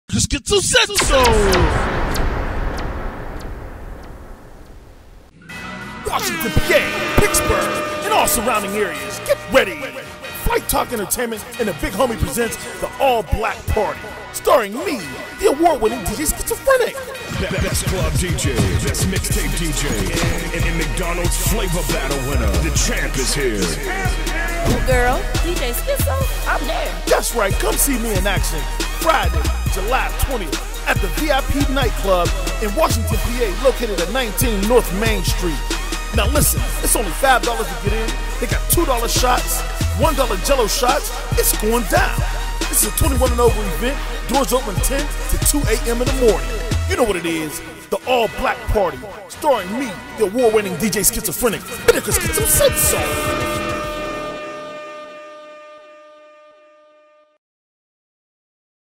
ALL BLACK PARTY STARRING THE AWARD WINNING DJ SCHIZO!!!!
FLIGHT TALK ENTERTAINMENT & THE BIG HOMIE PRESENTS THE ALL BLACK PARTY STARRING THE AWARD WINNING DJ SCHIZOPHRENIC. FRIDAY JULY 20, 2012 AT THE VIP NIGHTCLUB IN WASHINGTON, PA. $5.00 ADMISSION, $2.00 SHOTS, $1.00 JELLO SHOTS, 21 & OVER, DOORS OPEN 10PM-2AM. BE THERE CAUSE SCHIZO SAID SOOOOO!